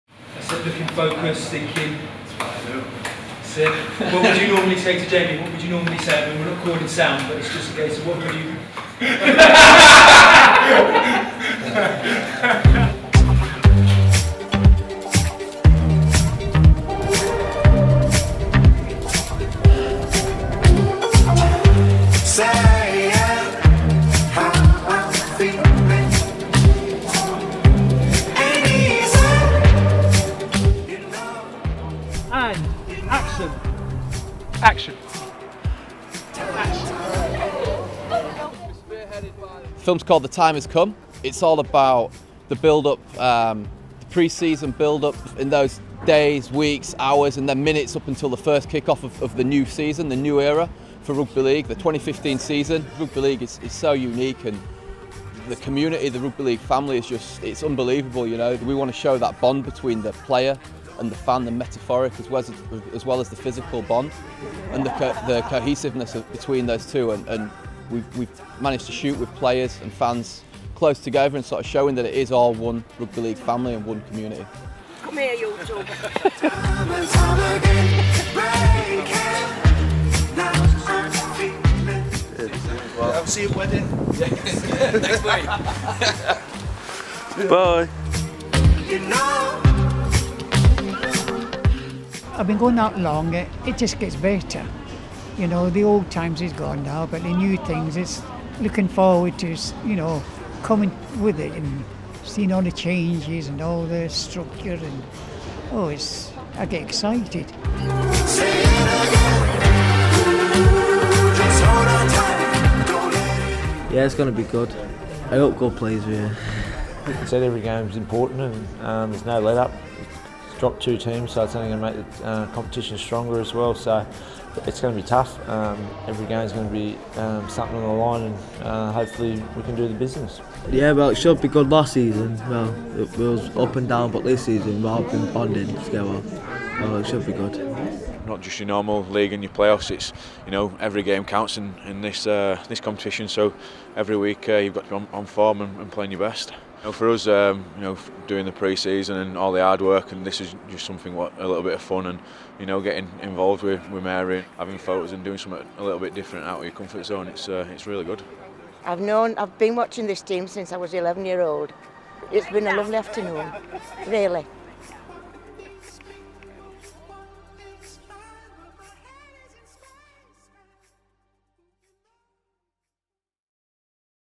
The Time Has Come - Behind the scenes...
Take a sneak peak behind the scenes of how 'The Time Has Come' Super League film ) came in to being. Hear from the film makers, fans and players that were involved...